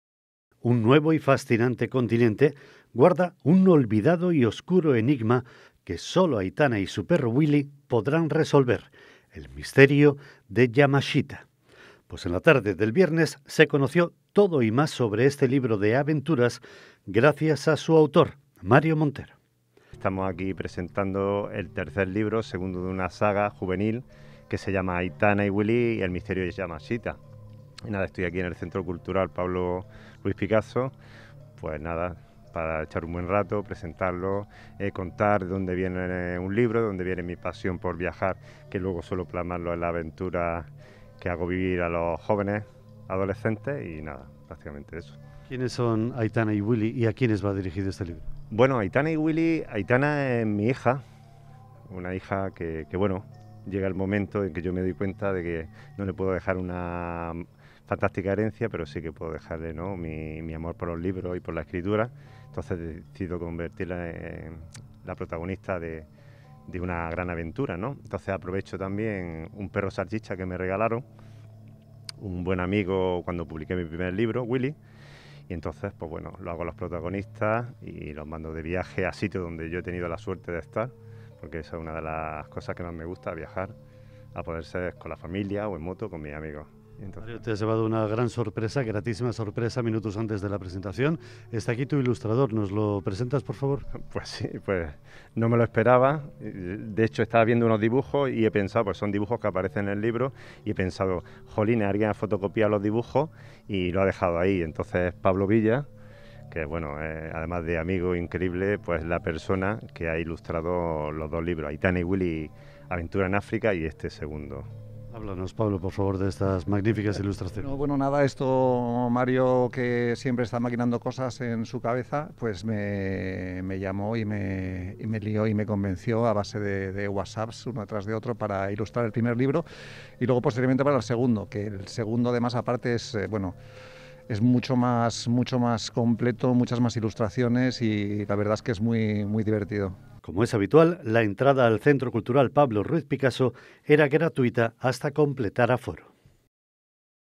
Este viernes se ha presentado el libro "Aitana y Willy. El misterio de Yamashita".
18/02/22
Su autor ha estado rodeado de amigos y el ilustrador en el Centro Cultural Pablo Ruiz Picasso.